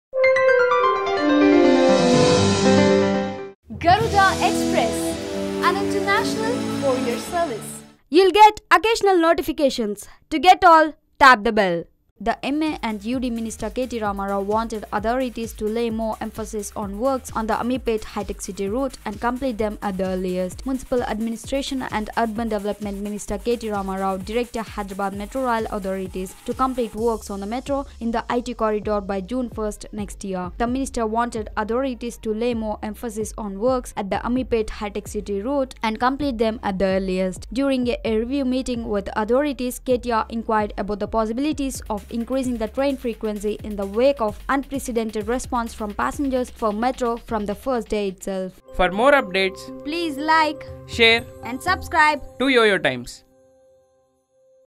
KTR Wants Metro Phase-2 Works in IT Corridor Completed By June ..!! | Telangana | HMR | YOYO Times
KTR Wants Metro Phase-2 Works in IT Corridor Completed By June ..!! Telangana,Hyderabad,HMR.
The MA&UD Minister K T Rama Rao wanted authorities to lay more emphasis on works on the Ameerpet-Hitech City route and complete them at the earliest. Municipal Administration and Urban Development Minister K T Rama Rao directed Hyderabad Metro Rail authorities to complete works on the Metro in the IT corridor by June 1 next year.

The Minister wanted authorities to lay more emphasis on works on the Ameerpet-Hitech City route and complete them at the earliest. During a review meeting with authorities, Rao enquired about the possibilities of increasing the train frequency in the wake of the unprecedented response from passengers for Metro from the first day itself. Responding to the Minister, Metro Rail authorities said the passenger response would be assessed till February. Depending on the situation, the train frequency would be increased.